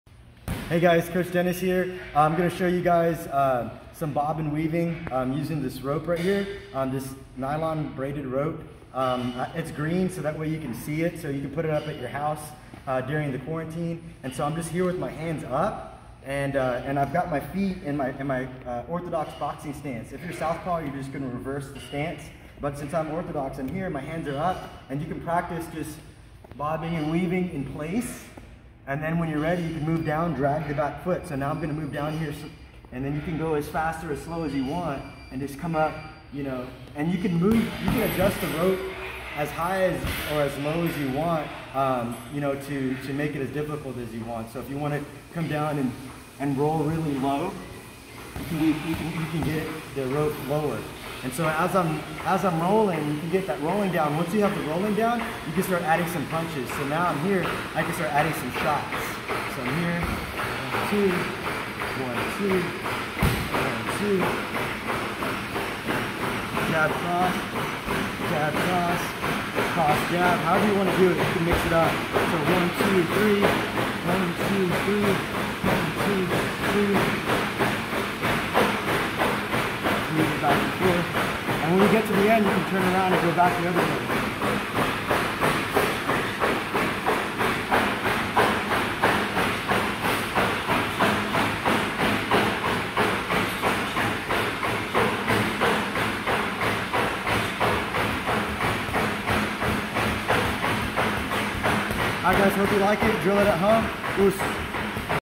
Bob and Weave drill using a rope. Roll drill.
Coach Dennis demonstrates a Bob and Weave drill using a rope. Boxing, MMA, Kickboxing, Muay Thai and Self-Defense. "Roll with the punches!"🥊